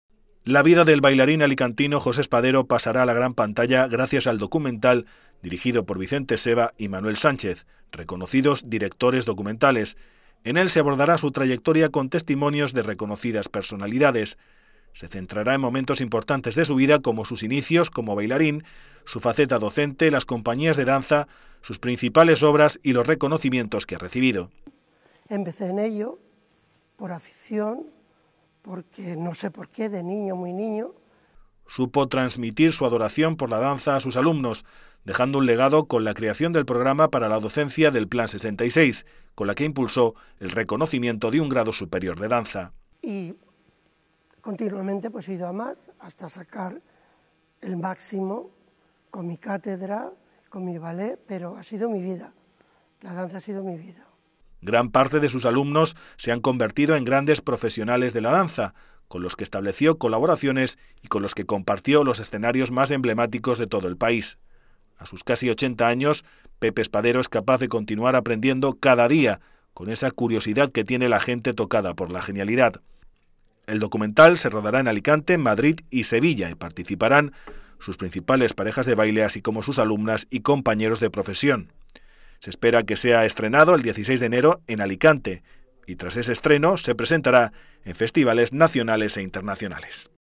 La vida del bailarín alicantino José Espadero llegará a la gran pantalla
La vida del bailarín alicantino José Espadero pasará a la gran pantalla gracias el documental dirigido por Vicente Seva y Manuel Sánchez, reconocidos directores documentales.

En él se abordará su trayectoria con testimonios de reconocidas personalidades. Se centrará en momentos importantes de su vida como sus inicios como bailarín, su faceta docente, las compañías de danza, sus principales obras y los reconocimientos que ha recibido.

Supo transmitir su adoración por la danza a sus alumnos, dejando un legado con la creación del programa para la docencia del plan 66, con la que impulsó el reconocimiento de un grado superior de danza.

Gran parte de sus alumnos se han convertido en grandes profesionales de la danza, con los que estableció colaboraciones y con los que compartió los escenarios más emblemáticos de todo el país. A sus casi 80 años, Pepe Espadero es capaz de continuar aprendiendo cada día con esa curiosidad que tiene la gente tocada por la genialidad.

El documental se rodará en Alicante, Madrid y Sevilla, y participarán sus principales parejas de baile , sus alumnas y compañeros de profesión del bailarín. Se espera que sea estrenado el 16 de enero en Alicante y, tras su estreno, se presentará en festivales nacionales e internacionales.